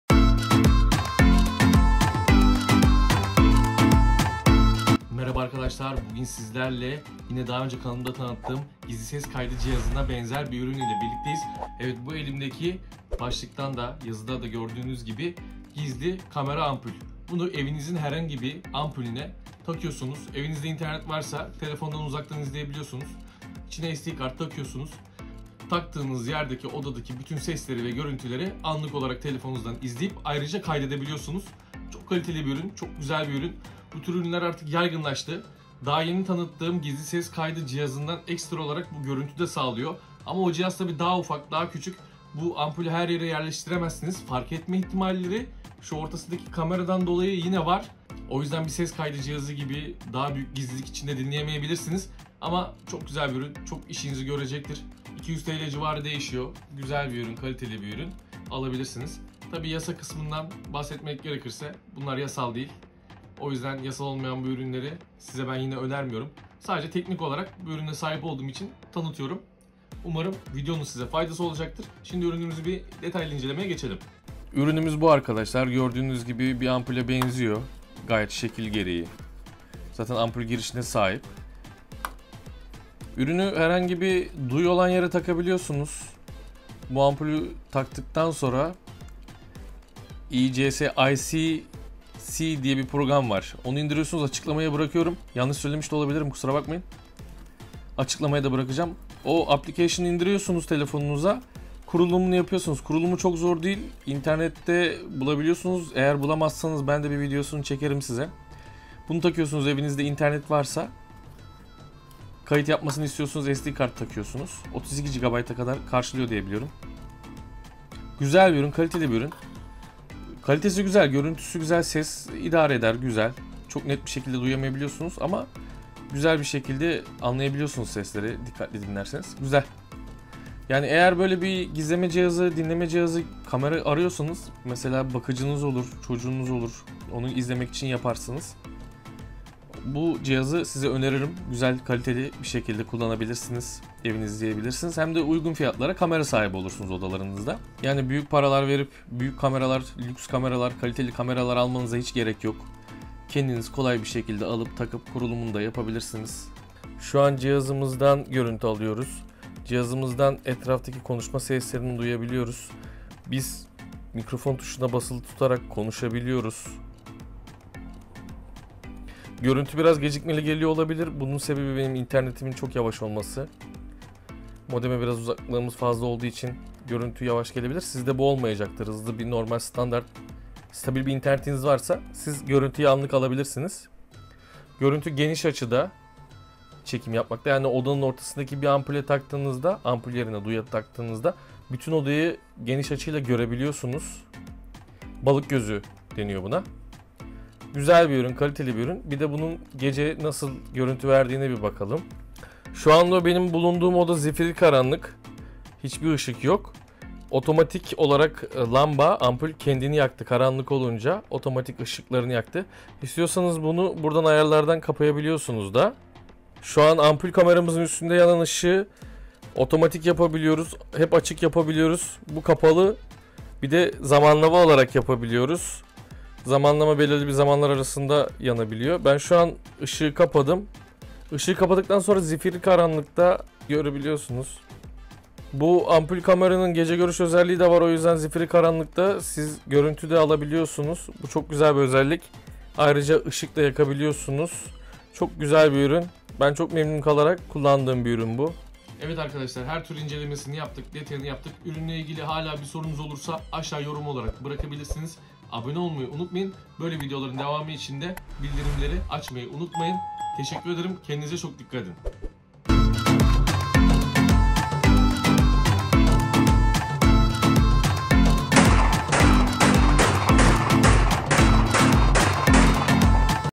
TÜRKİYE'NİN EN KALİTELİ GİZLİ AMPUL CASUS KAMERASI ( UYGUN FYATLI ) İNCELEME
DİREK SORU VE BİLGİ İÇİN INSTAGRAM HESABIMI TAKİP EDİP MESAJ ATABİLİRSİNİZ ! ( TAKİP EDİLMEDEN ATILAN MESAJLAR GÖZÜKMEMEKTEDİR )

Türkiyenin En Kaliteli Gizli Ampul Casus Kamerası ( UYGUN FİYATLI )

Bu ürün hem gizlilik hemde kaliteli bir güvenlik sunarak telefonunuzdan veya diğer akıllı cihazlardan uygulama ile evinizi iş yerinizi izleyebileceksiniz.

DAHA UYGUNA VE GELİŞMİŞ ÖZELLİKLİ ALİEXPRESS LİNK : ( FARKLI APP (UYGULAMA) İLE KULLANILIR )

30 GÜN ŞARJLI GİZLİ KAYIT CİHAZI